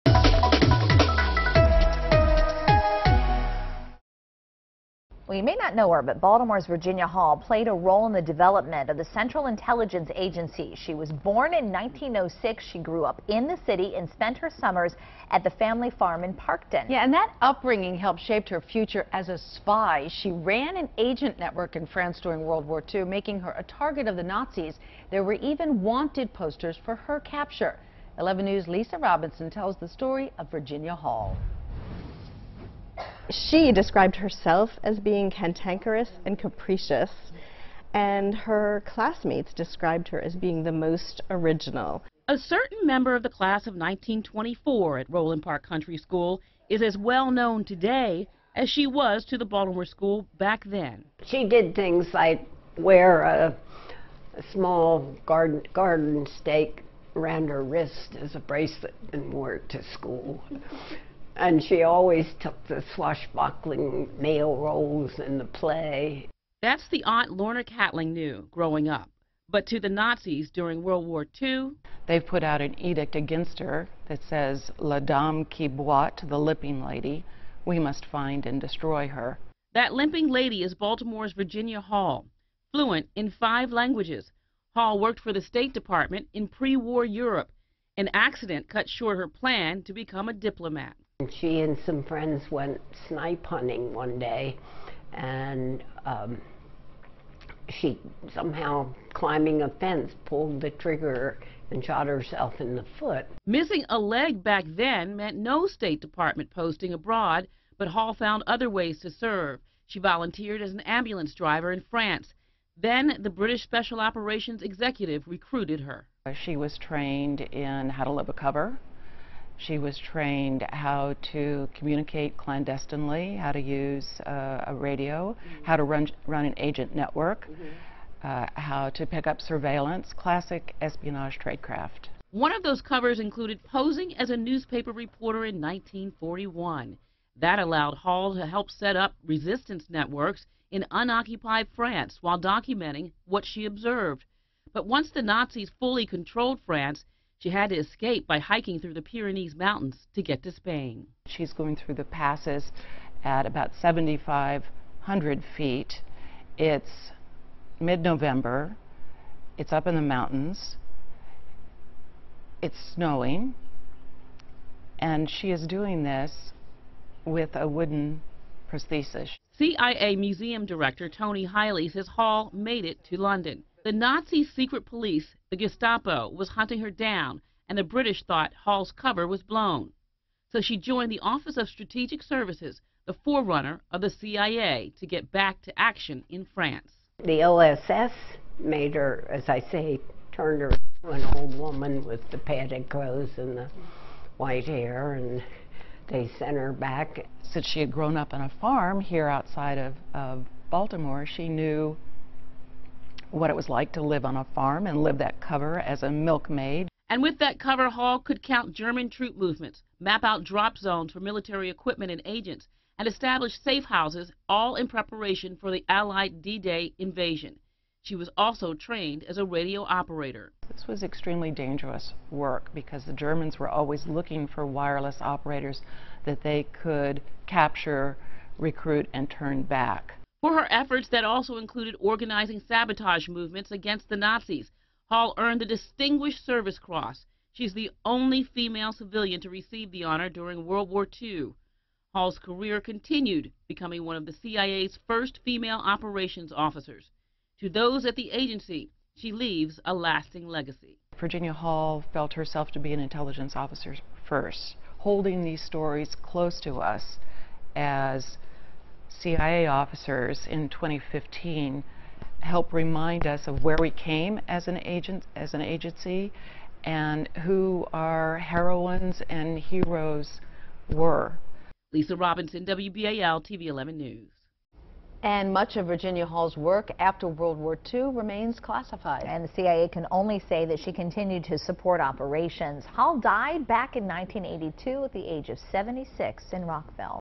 Virginia Hall's critical role as an American spy
Virginia Hall's upbringing in Baltimore helped shape her future as a spy. The Central Intelligence Agency is the nation's clearinghouse for foreign intelligence.